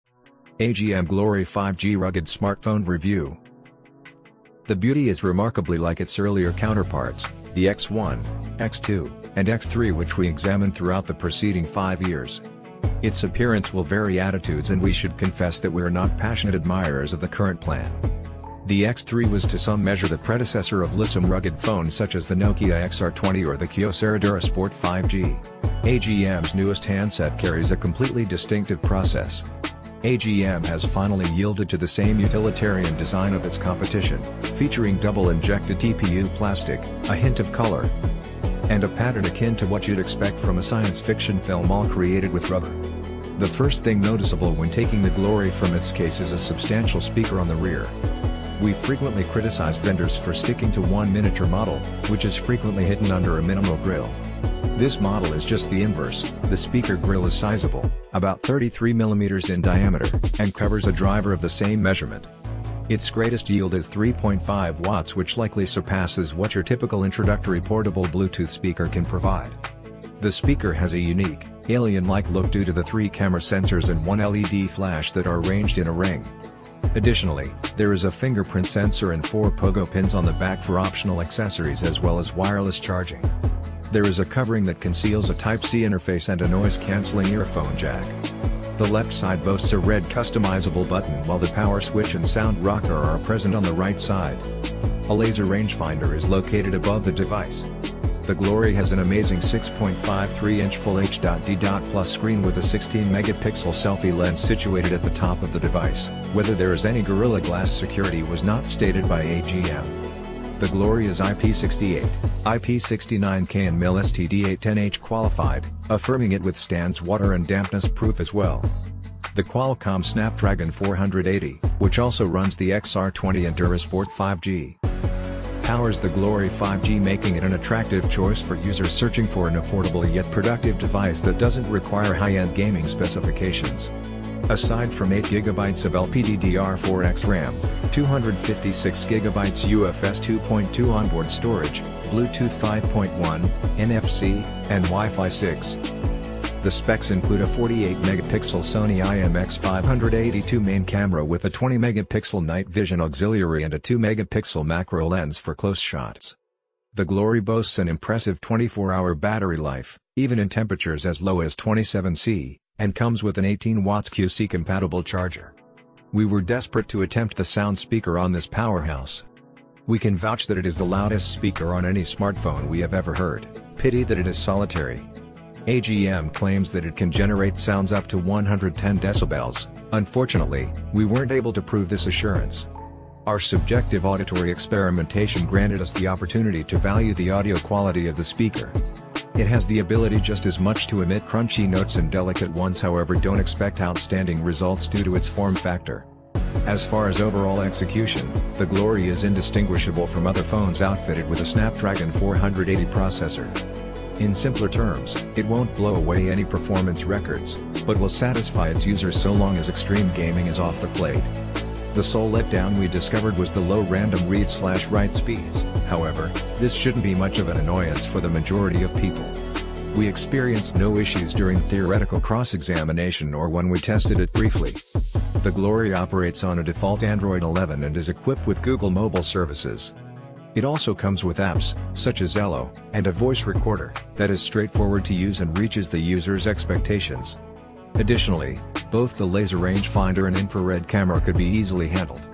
Agm Glory 5G Rugged Smartphone Review - Built Tough for the Outdoors
Amazon Product Name : Agm Glory 5G Rugged Smartphone

Looking for a rugged and durable smartphone that can handle even the toughest conditions? Check out the AGM Glory 5G! In this video review, we'll explore the features and capabilities of this top-rated rugged smartphone, including its 5G connectivity, waterproof design, and long battery life. Tune in to learn more about this top-rated rugged smartphone and how it can transform the way you work and play!